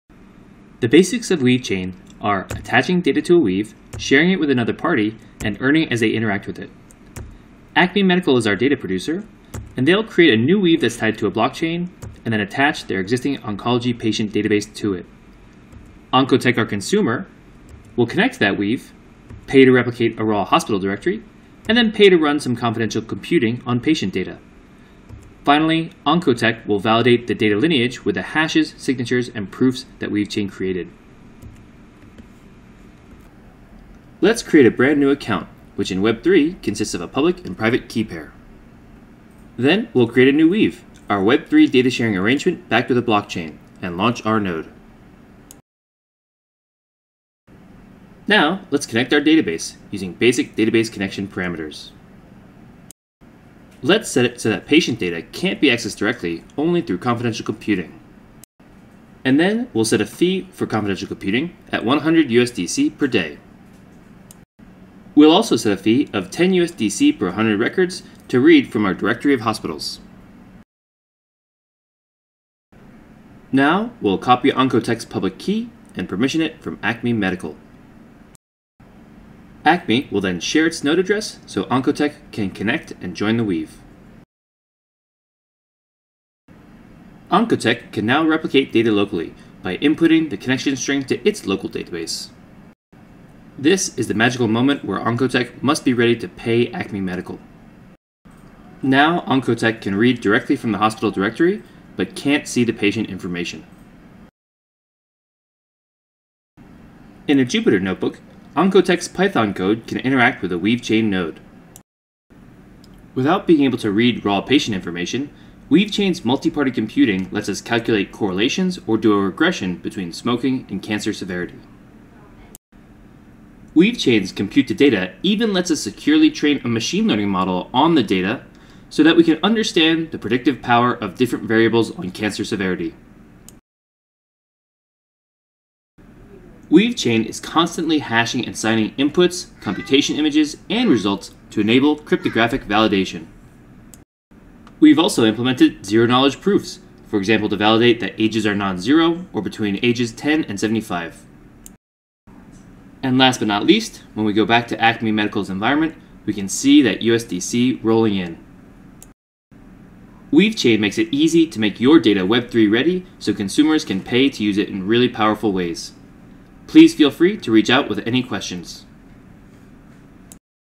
Weavechain Basics: Attach, Share, Earn
Learn how easy it is to use Weavechain to make your data Web3 ready, so consumers can pay to use it in really powerful ways.

Chapters:
00:00 Overview
00:32 Setting up a Weave
00:46 Attaching to DB, Sharing
01:11 Connecting to Weave
01:25 Replicating Raw Data
01:45 Confidential Computing
02:14 Hashing, Signing, ZK Proofs
02:39 Thank You!